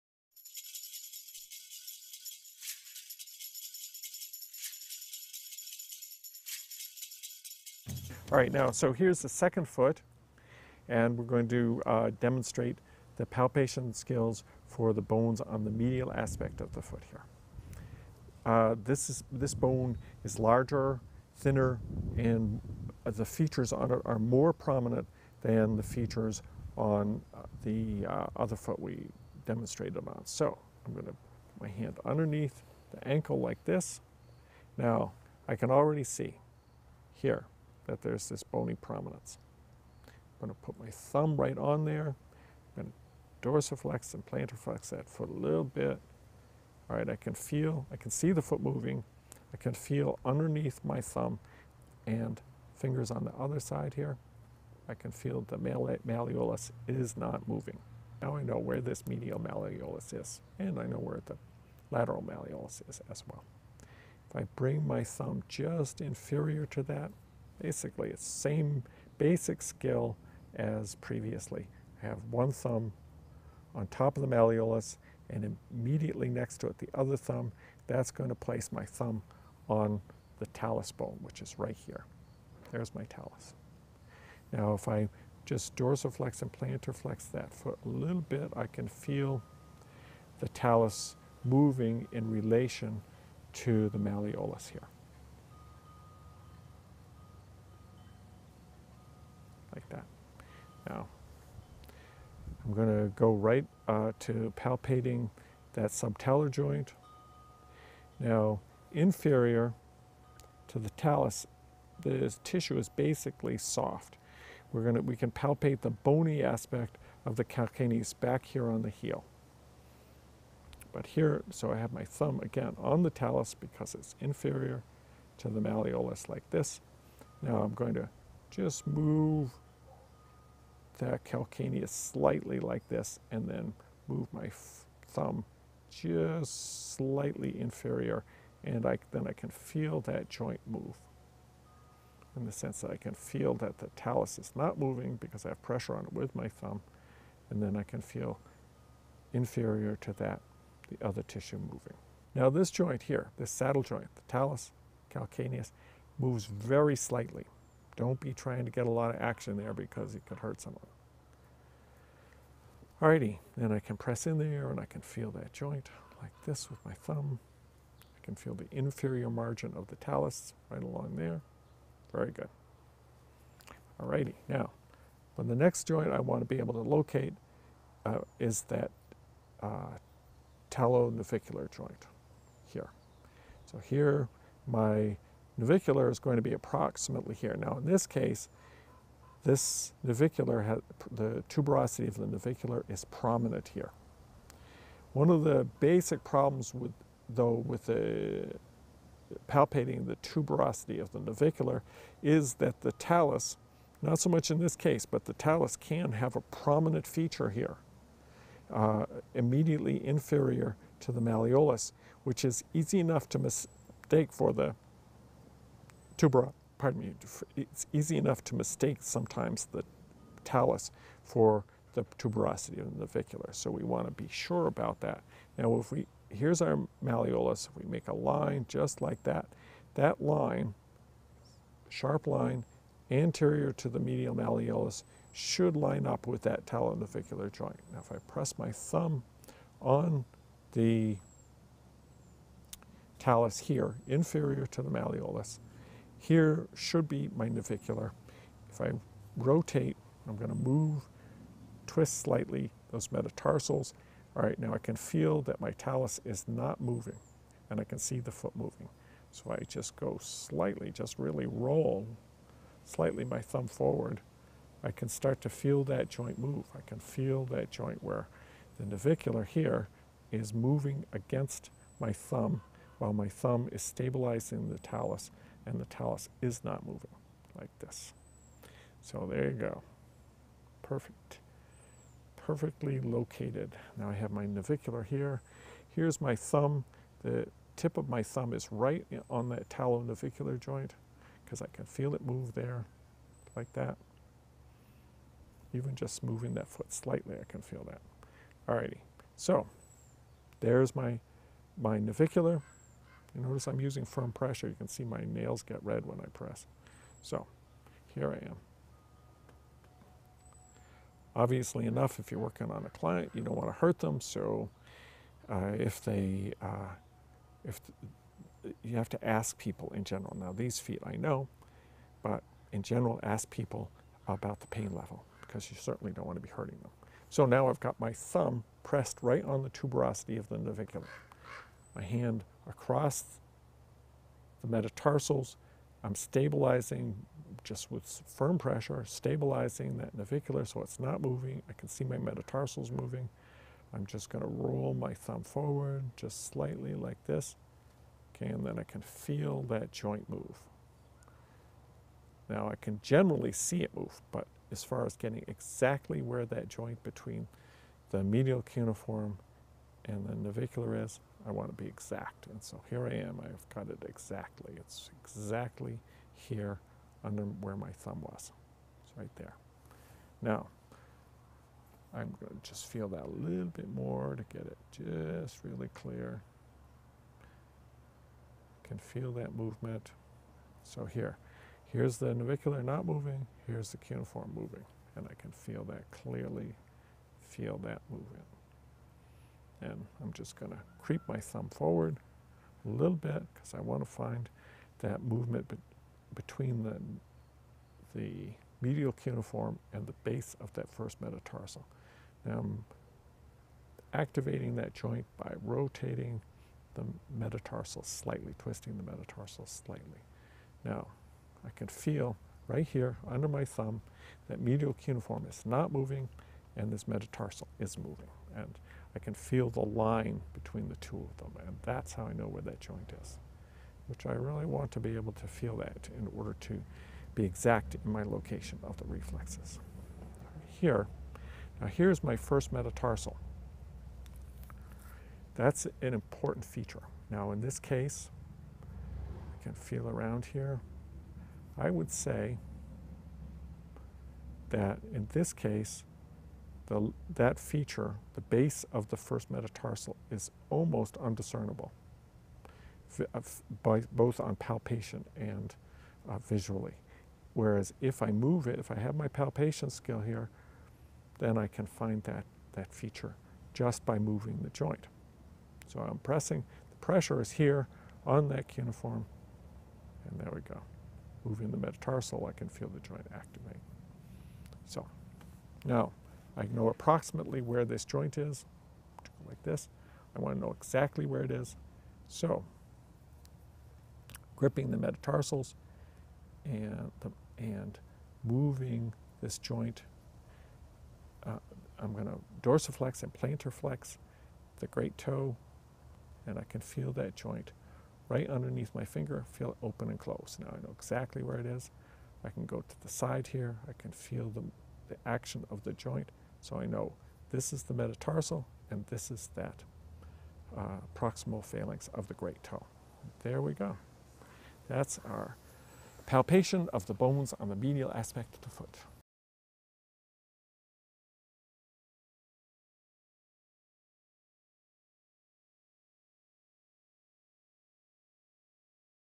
Live foot palpation Demo #2
This is a video #2 showing a demonstration of  skills for palpation of the bones of the medial aspect of the foot of a live subject.